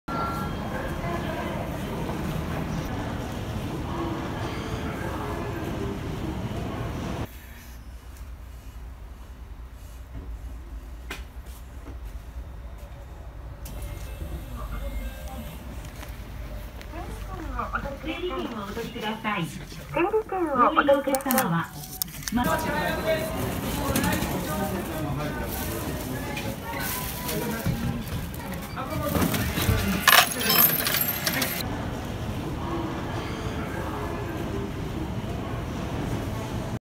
飯田線 新時代! スピード精算しませんか？
飯田線も新時代が来ました。運賃箱でスピード精算出来ます。